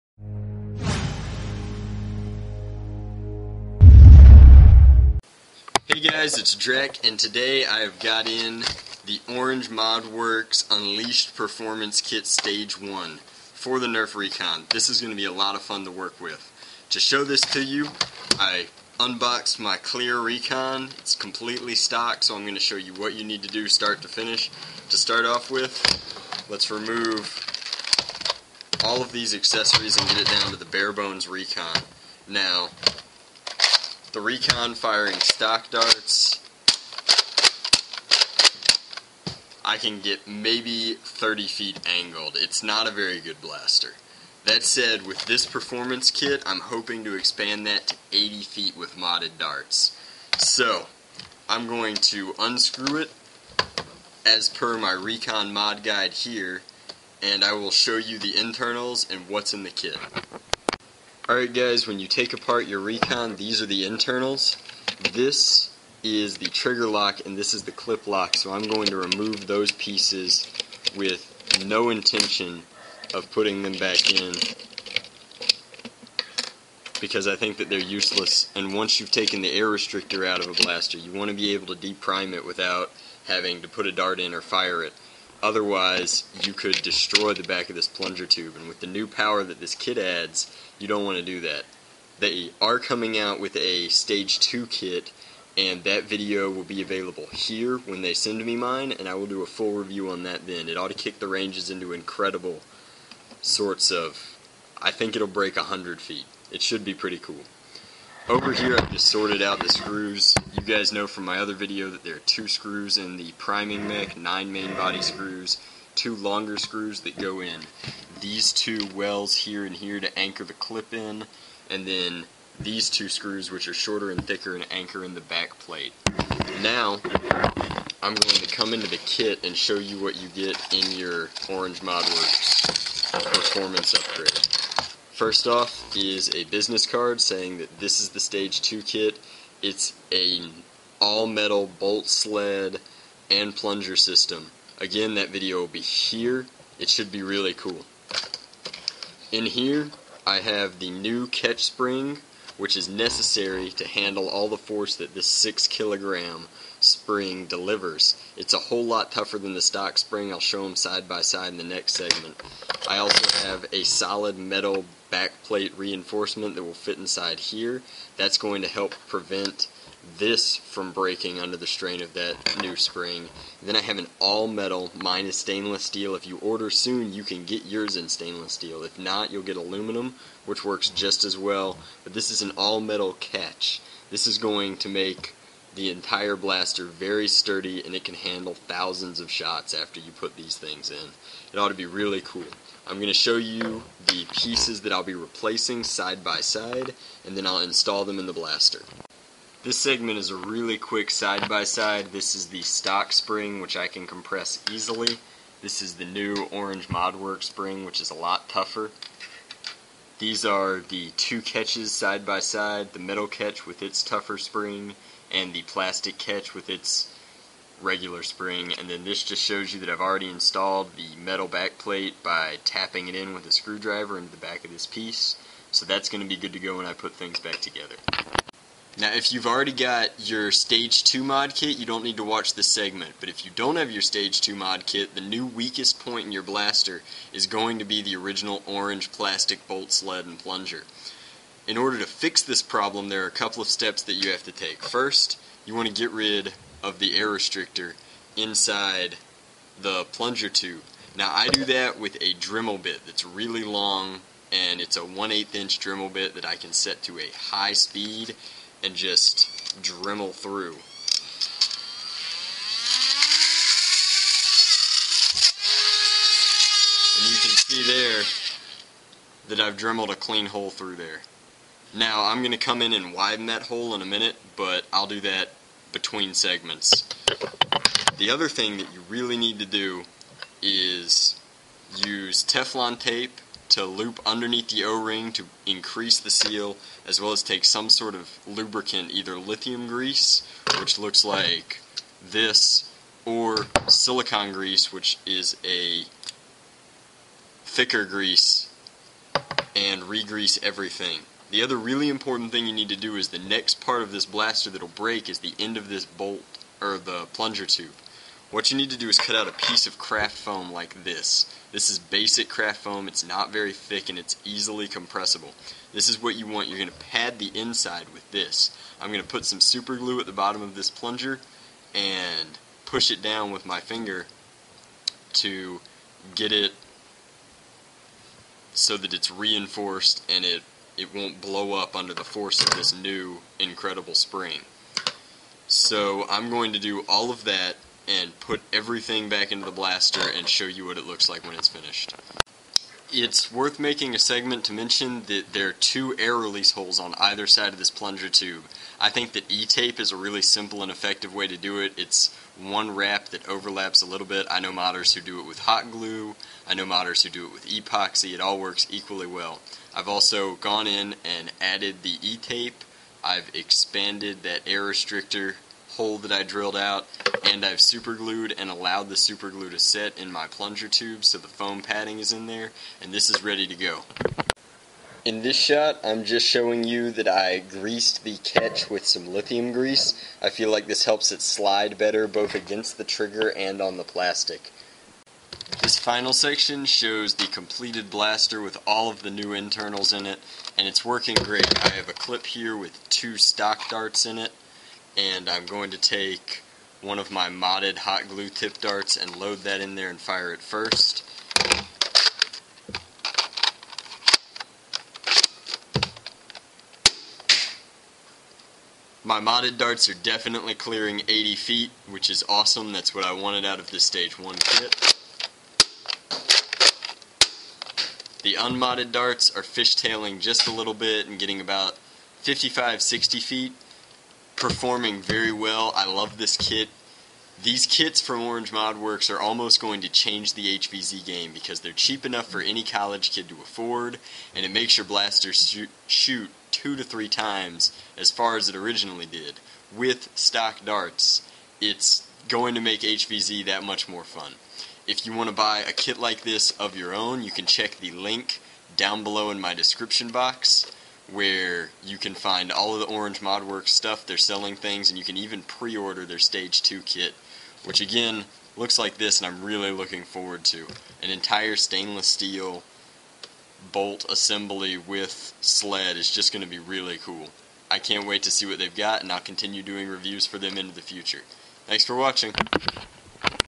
Orange Mod Works Recon Kit #1 (Review and How To Install Guide)
In this video, I show you how to install the Orange Mod Works Recon Kit 1.   It is rather nice.